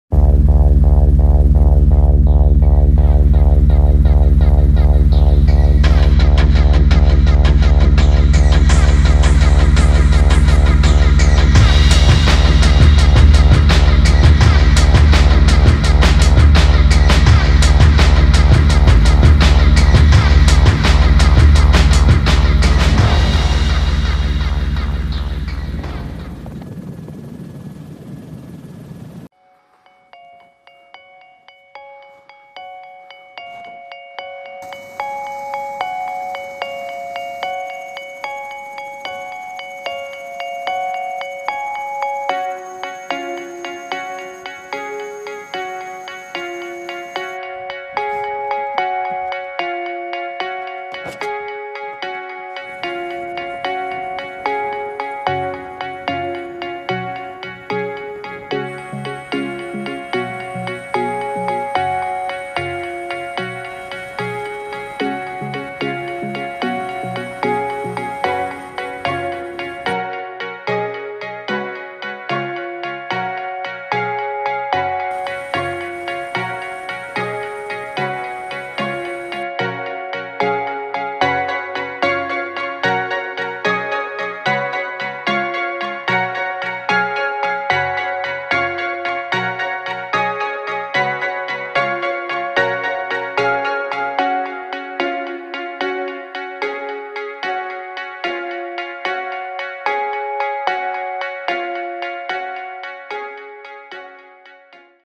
Австралийские спасатели сбросили тонну овощей для пострадавших от пожаров животных
Служба охраны дикой природы национального парка Блу-Маунтинс в Австралии в понедельник сбросила с вертолета более тысячи килограммов моркови и сладкого картофеля для животных, пострадавших от лесных пожаров.
Источник: ruptly.tv, NSW National Parks and Wildlife Service
Подготовил: Евгений Жура